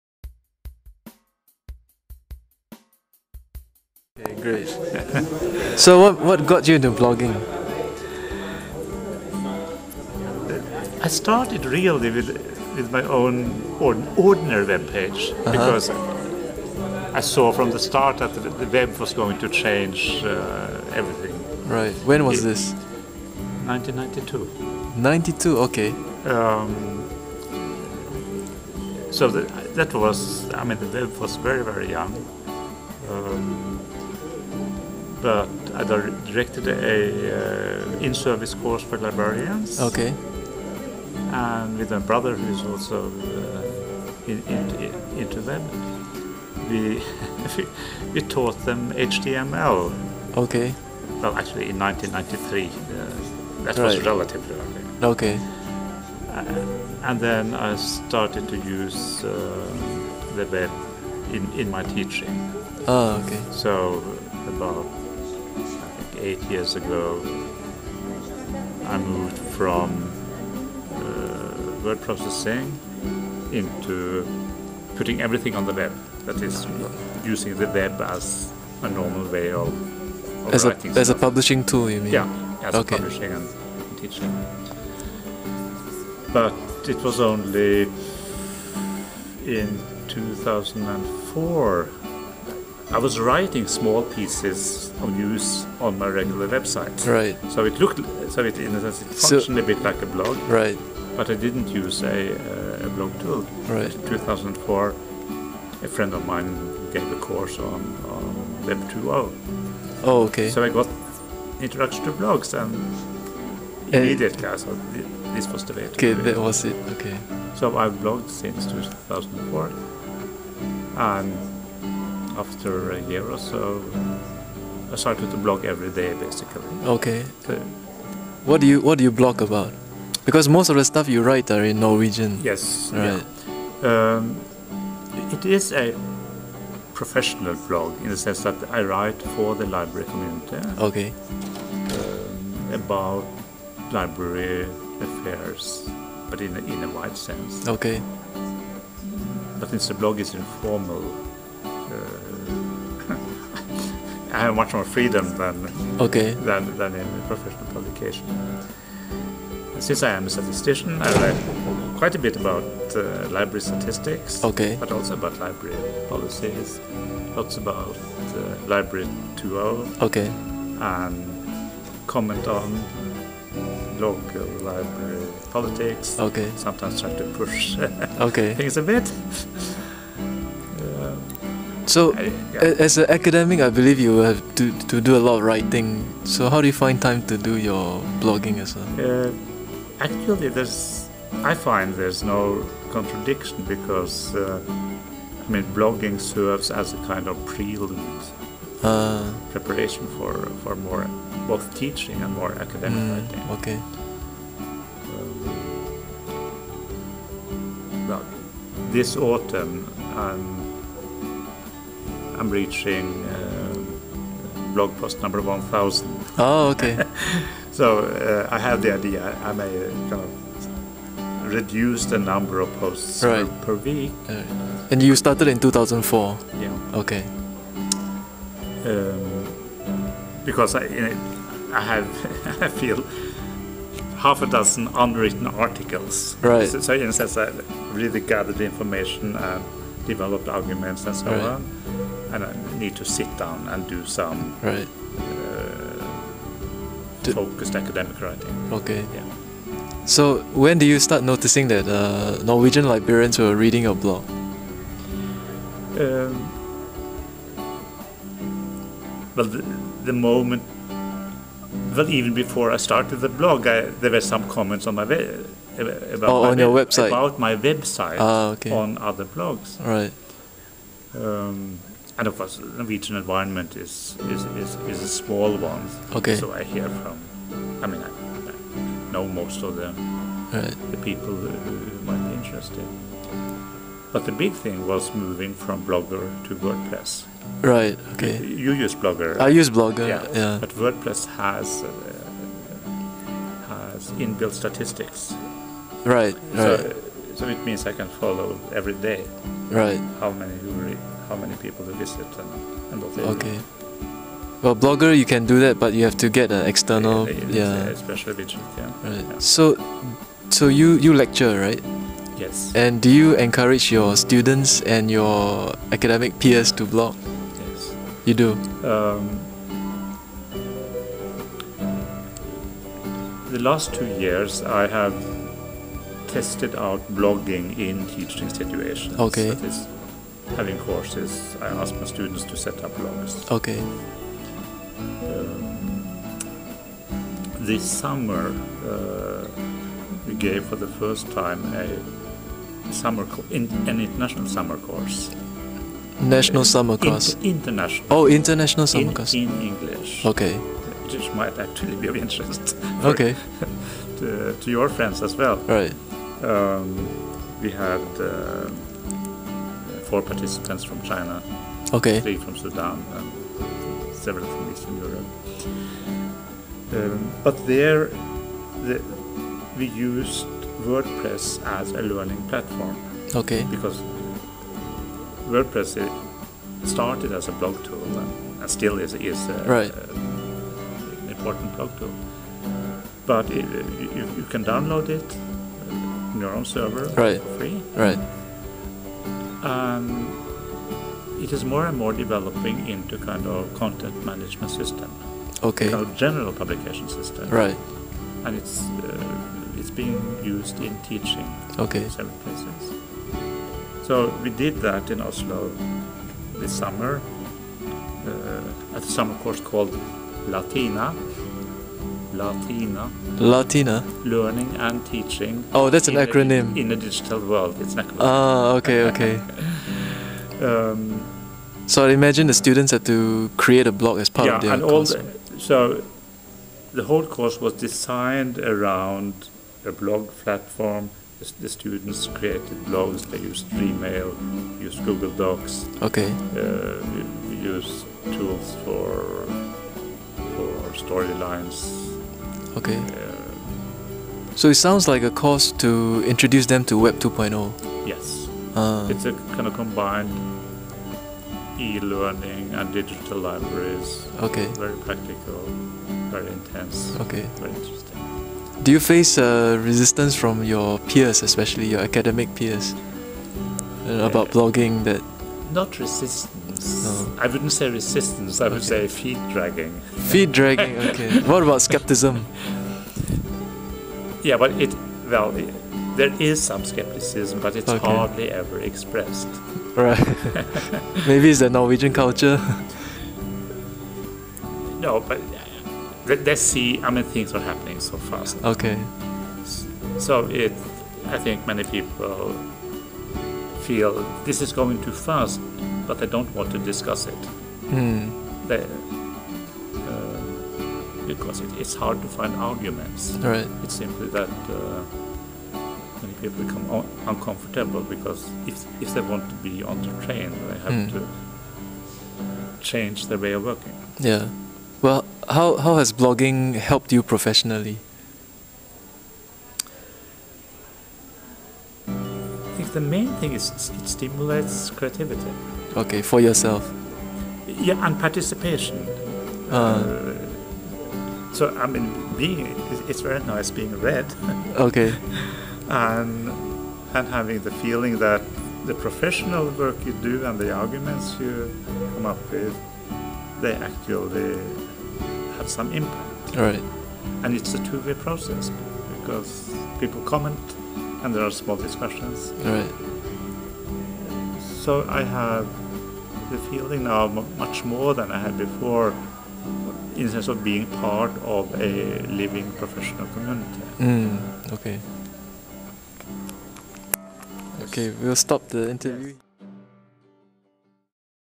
Interview with Pliny the Librarian
Quebec City, Canada. IFLA 2008 conference. An impromptu interview with Tord Høivik. Associate Professor of Library and Information Science at Oslo University College, aka Pliny the Librarian - We met face-to-face for the first time, while at the IFLA 2008 conference. I asked if he was willing to be interviewed and he gamely said yes. Except for the intro/ end title and music, the entire video has not been unedited.